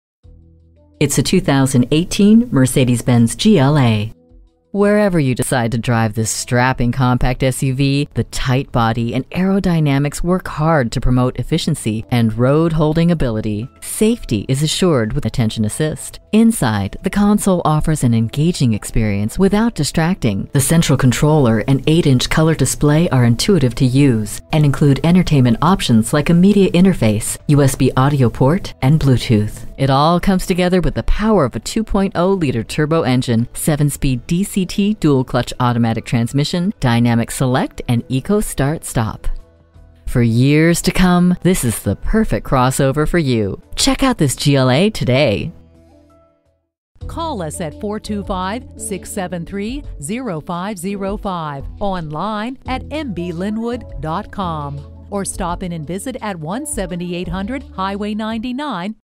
New 2018 Mercedes-Benz GLA Lynnwood WA Seattle, WA #28630 - SOLD
Year: 2018
Make: Mercedes-Benz
Model: GLA
Trim: GLA 250 4MATIC
Engine: 4 Cyl. 2.0 L
Transmission: Automatic
Color: Cirrus White
Interior: Black
Stock #: 28630
VIN: WDCTG4GB1JJ472334
WHEEL LOCKS, ILLUMINATED STAR, CONVENIENCE PACKAGE -inc: Garage Door Opener Auto-Dimming Rearview & Driver-Side Mirrors, COMPASS -inc: Display in rearview mirror, PREMIUM PACKAGE -inc: KEYLESS GO Heated Front Seats Blind Spot Assist SIRIUSXM Satellite Radio HANDS-FREE ACCESS, SMARTPHONE INTEGRATION -inc: Android Auto Apple CarPlay Smartphone Integration (14U), PANORAMA SUNROOF, ALUMINUM TRIM, WHEELS: 18 TWIN 5-SPOKE ALLOY, RADIO: HARMAN/KARDON PREMIUM SOUND SYSTEM -inc: 500 watt amplifier and Digital Dynamic Volume Control, AMBIENT LIGHTING (12 COLORS), FULL LED HEADLAMPS, LED LOGO PROJECTOR

Address:
17800 Highway 99